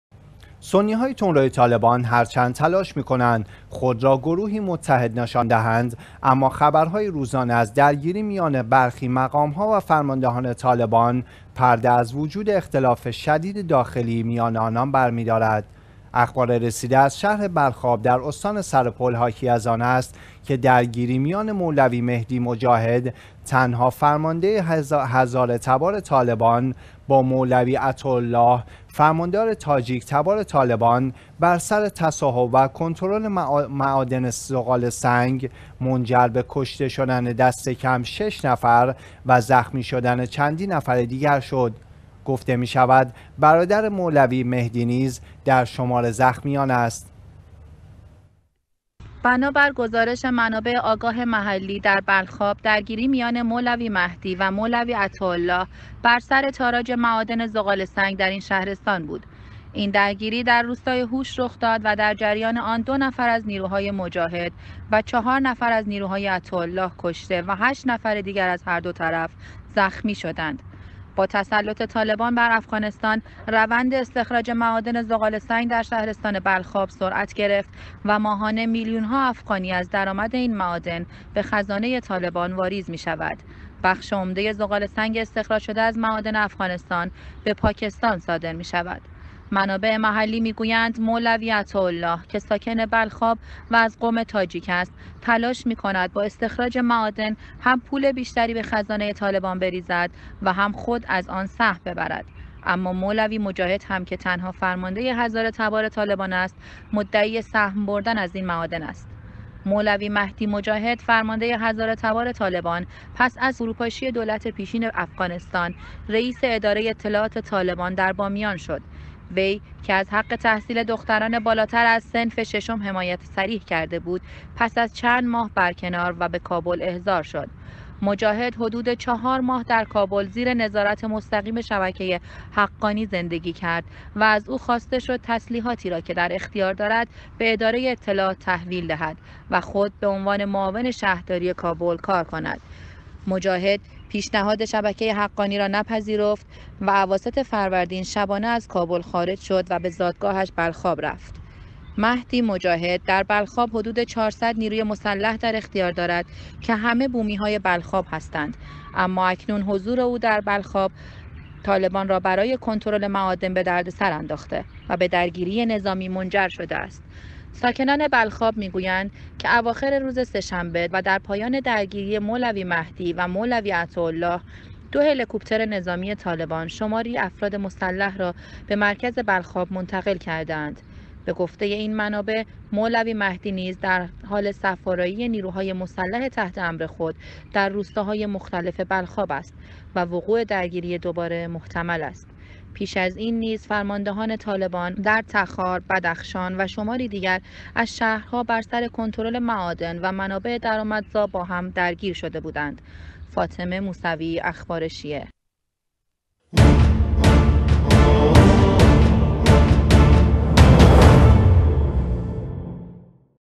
۶ کشته و چندین زخمی نتیجه درگیری فرماندهان طالبان با یکدیگر
سنی‌های تندروی طالبان هر چند تلاش می‌کنند خود را گروهی متحد نشان دهند ، اما خبرهای روزانه از درگیری میان برخی مقام‌ها و فرماندهان طالبان ، پرده از وجود اختلاف شدید داخلی میان آنان برمی‌دارد.

اخبار رسیده از شهر بلخاب در استان سرپل حاکی از آن است که درگیری میان مولوی مهدی مجاهد تنها فرمانده هزاره‌تبار طالبان با مولوی عطاءالله فرماندار تاجیک‌تبار طالبان بر سر تصاحب و کنترل معادن زغال سنگ ، منجر به کشته شدن دست کم ۶ نفر و زخمی شدن چندین نفر دیگر شد ، گفته می‌شود برادر مولوی مهدی نیز در شمار زخمیان است.

بنا بر گزارش منابع آگاه محلی در بلخاب ، درگیری میان مولوی مهدی و مولوی عطاءالله ، بر سر تاراج معادن زغال‌ سنگ در این شهرستان بود.
این درگیری در روستای هوش رخ داد و در جریان آن ، ۲ نفر از نیروهای مجاهد و ۴ نفر از نیروهای عطاءالله کشته و ۸ نفر دیگر از هر دو طرف زخمی شدند.

با تسلط طالبان بر افغانستان روند استخراج معادن زغال سنگ در شهرستان بلخاب سرعت گرفت و ماهانه میلیون‌ها افغانی از درآمد این معادن به خزانه طالبان واریز می‌شود. بخش عمده زغال سنگ استخراج‌شده از معادن افغانستان به پاکستان صادر می‌شود.

منابع محلی می‌گویند مولوی عطاءالله که ساکن بلخاب و از قوم تاجیک است ، تلاش می‌کند با استخراج معادن هم پول بیشتری به خزانه طالبان بریزد و هم خود از آن سهم ببرد ، اما مولوی مجاهد هم که تنها فرمانده هزاره‌تبار طالبان است ، مدعی سهم‌ بردن از این معادن است.

مولوی مهدی مجاهد ، فرمانده هزاره‌تبار طالبان ، پس از فروپاشی دولت پیشین افغانستان ، رئیس اداره اطلاعات طالبان در بامیان شد.
وی که از حق تحصیل دختران بالاتر از صنف ششم حمایت صریح کرده بود ، پس از چند ماه ، برکنار و به کابل احضار شد.

مجاهد حدود چهار ماه در کابل زیر نظارت مستقیم شبکه حقانی زندگی کرد و از او خواسته شد تسلیحاتی را که در اختیار دارد ، به اداره اطلاعات تحویل دهد و خود به عنوان معاون شهرداری کابل کار کند. مجاهد پیشهاد شبکه حقانی را نپذیرفت و اواسط فروردین ، شبانه از کابل خارج شد و به زادگاهش بلخاب رفت.

مهدی مجاهد در بلخاب حدود ۴۰۰ نیروی مسلح در اختیار دارد که همه بومی‌های بلخاب هستند اما اکنون حضور او در بلخاب طالبان را برای کنترل معادن به دردسر انداخته و به درگیری نظامی منجر شده است.

ساکنان بلخاب می‌گویند که اواخر روز سه‌شنبه و در پایان درگیری مولوی مهدی و مولوی عطاءالله ، دو هلی‌کوپتر نظامی طالبان شماری افراد مسلح را به مرکز بلخاب منتقل کرده‌اند. به گفته این منابع ، مولوی مهدی نیز در حال صف‌آرایی نیروهای مسلح تحت‌امر خود در روستاهای مختلف بلخاب است و وقوع درگیری دوباره محتمل است.

پیش از این نیز فرماندهان طالبان در تخار، بدخشان و شماری دیگر از شهرها بر سر کنترل معادن و منابع درآمدزا با هم درگیر شده‌ بودند.